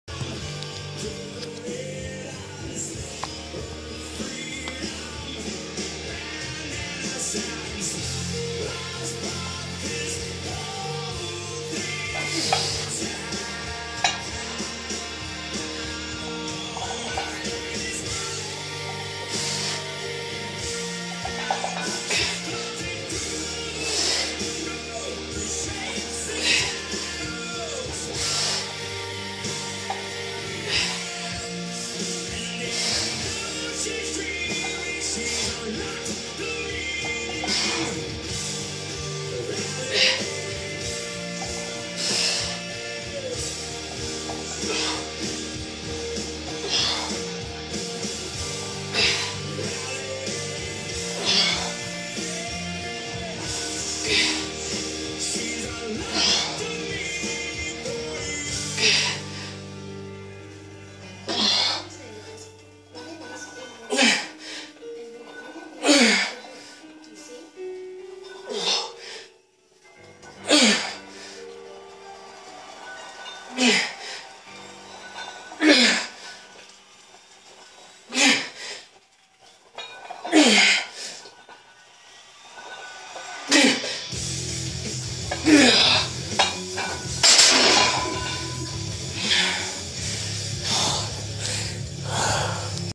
Zercher squat 3
Third set. Bit of a grinder. Lower abs may have gotten tired on this set and extended in lower back as the set went on. Maybe i'm overanalyzing. ..